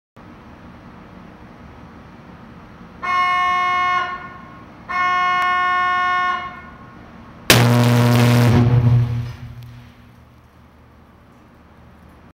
40 Cal Arc Flash Hood Test Video
Arc flash test for NSA's 40 cal/cm² Arc Flash DuPont™ Protera® hood (left) compared to competitor (right).